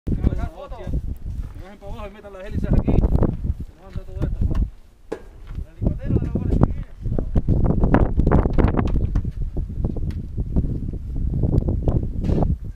Retirando la nieve en el Teleférico del Teide tras las primeras nevadas del 2014
Breve vídeo donde se pueden apreciar las vistas nevadas del Teide tras las primeras nevadas de este 2014.